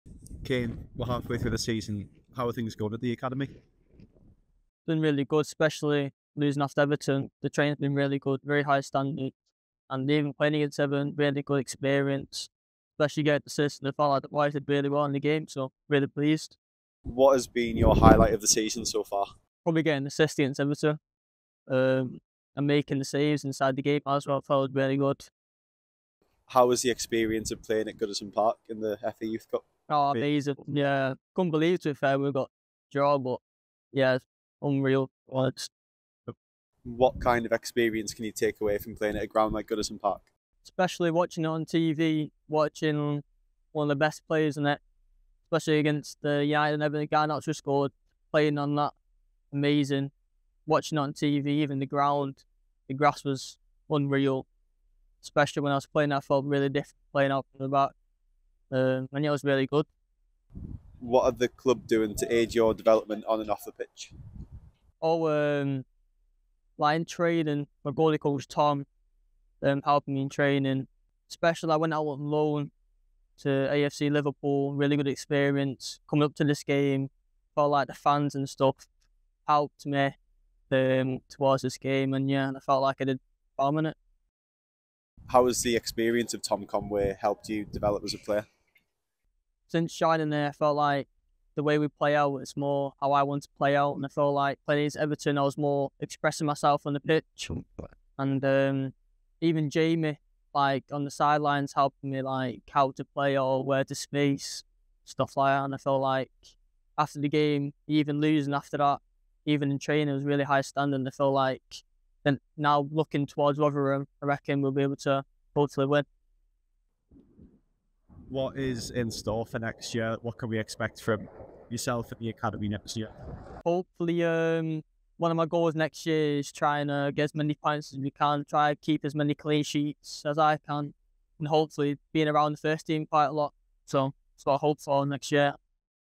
Academy December Update | Kane Mavrantziotis-Bove
Goalkeeper for our Academy Scholars Kane Mavrantziotis-Bove shared his thoughts on his first-year at Stockport Sports Village, the experience of playing at Goodison Park and how the staff have helped him develop, both on and off the pitch.